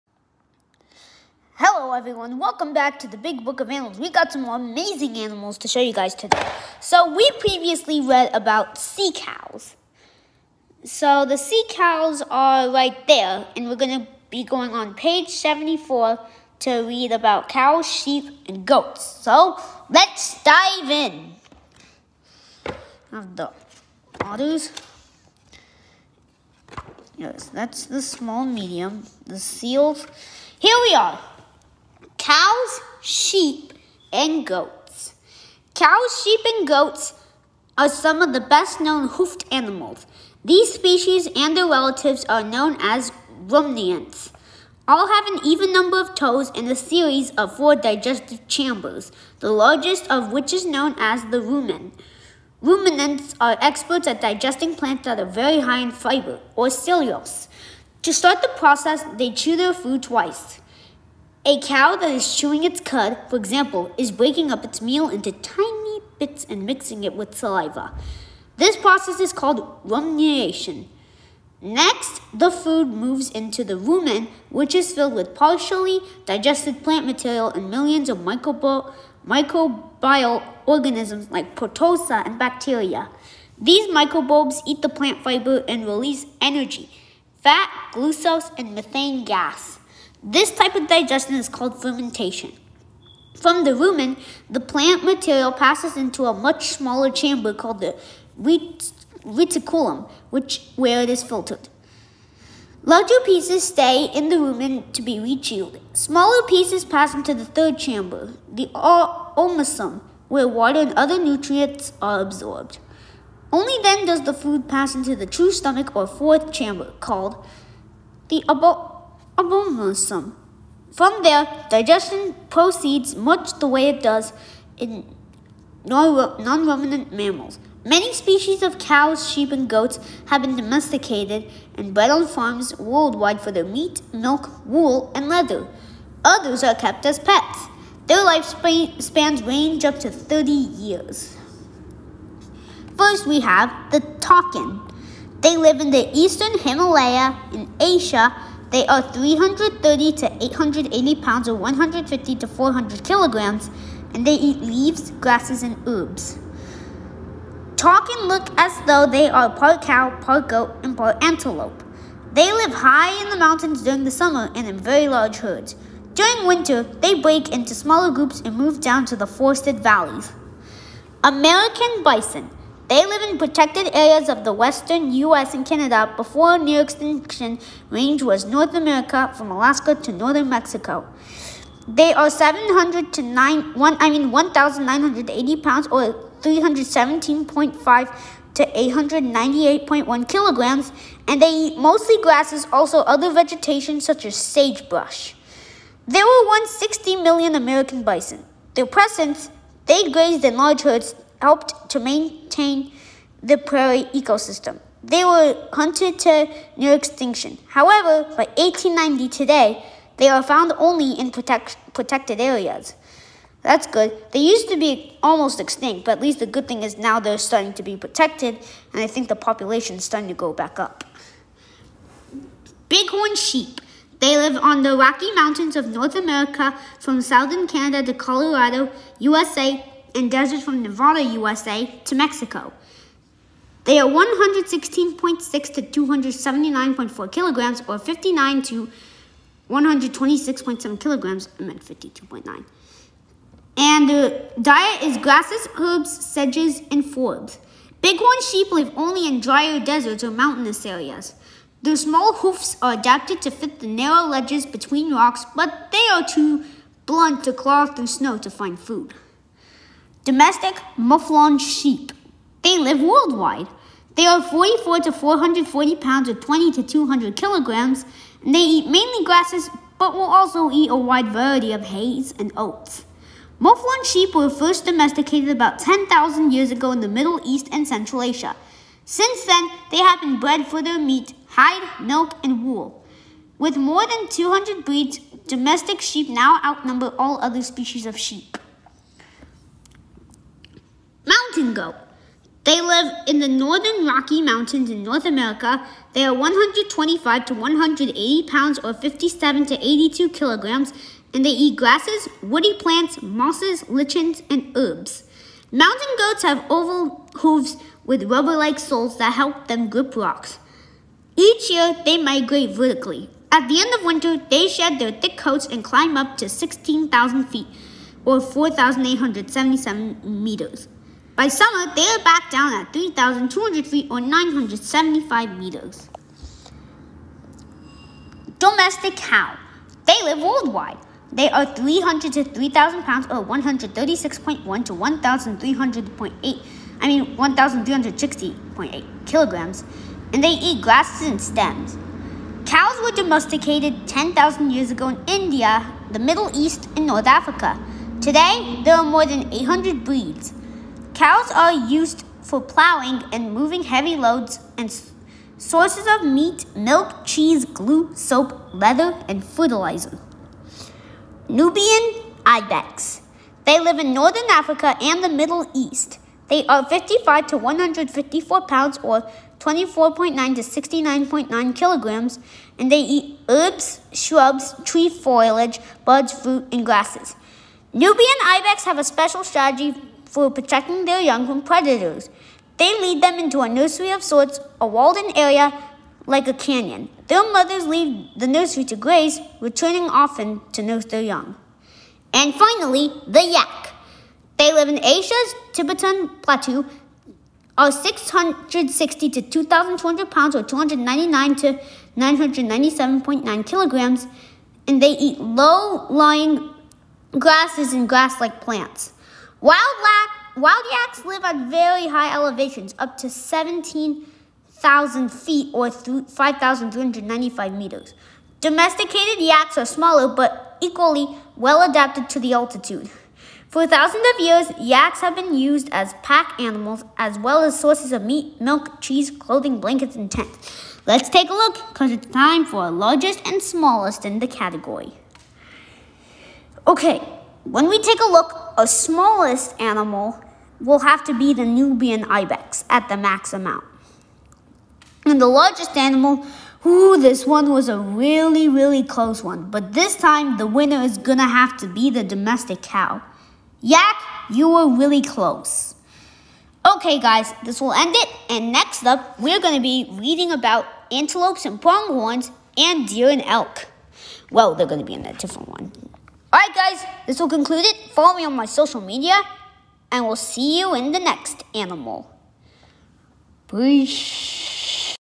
National Geographic Animal Encyclopedia - Learn about Cows, Sheep, and Goats!
Ahoy! Today we learn about cows sheep and goats. The most common species we learned were wild animals but some have also been domesticated to farms for meat milk and jobs like carrying heavy loads on a wagon.